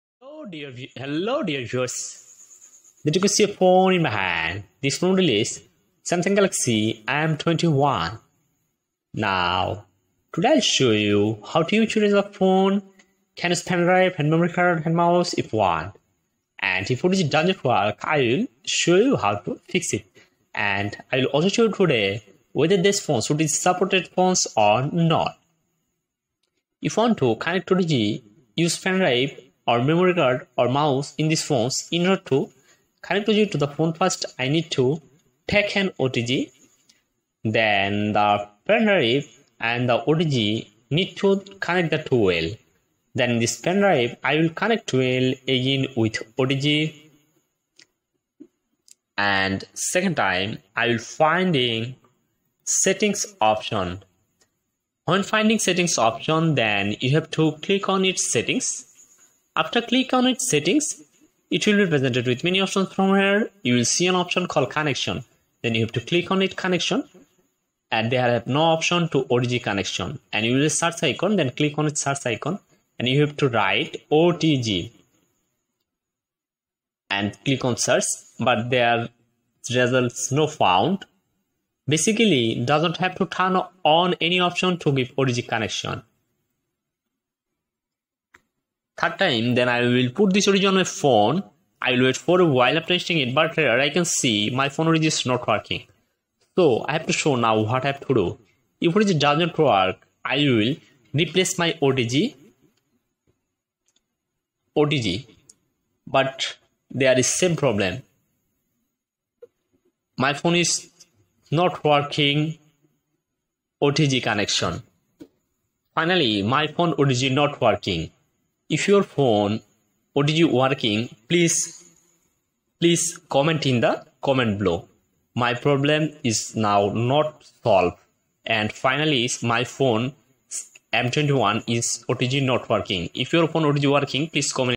Samsung galaxy M21 Otg Support Setting // Otg not working problem
You can see a phone this phone model is Samsung galaxy m21. now today i will show you this phone otg connection work or not work